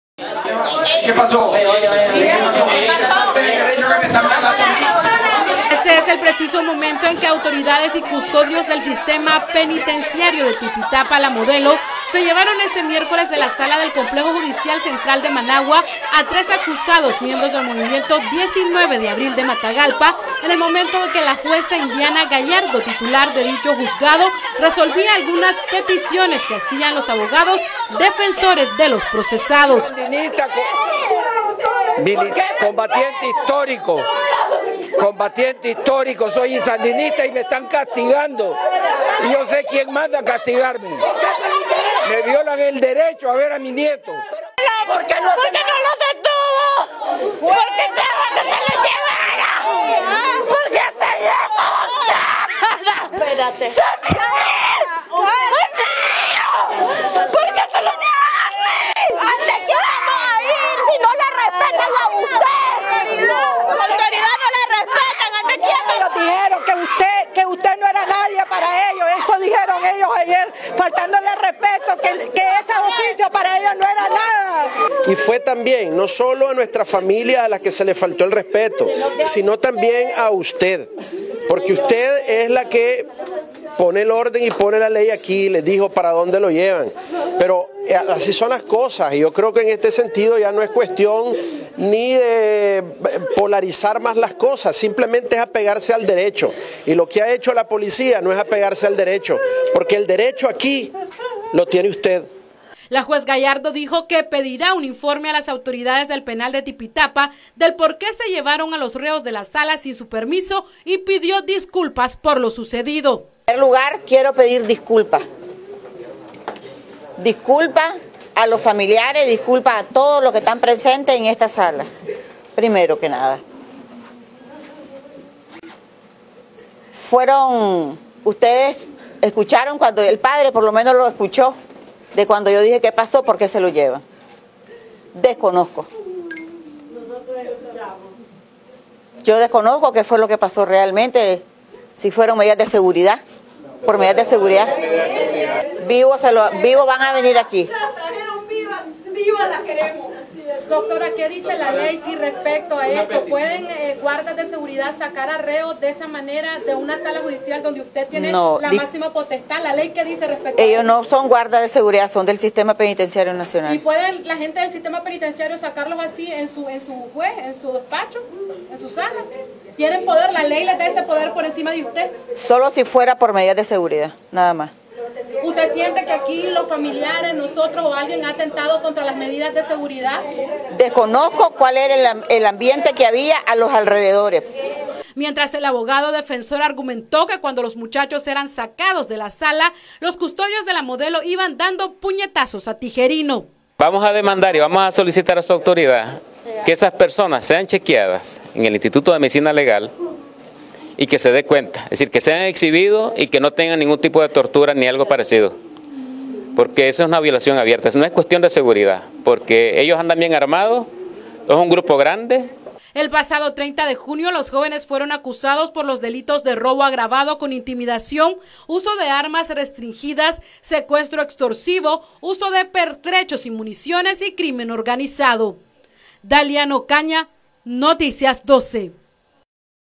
La audiencia inicial a los cinco jóvenes del Movimiento 19 de Abril, originarios de Matagalpa.
La audiencia inicial a los cinco jóvenes del Movimiento 19 de Abril, originarios de Matagalpa se suspendió el pasado miércoles 17 de Julio en los Juzgados de Managua.
Los jóvenes fueron retirados de manera intempestiva y violenta de la sala judicial.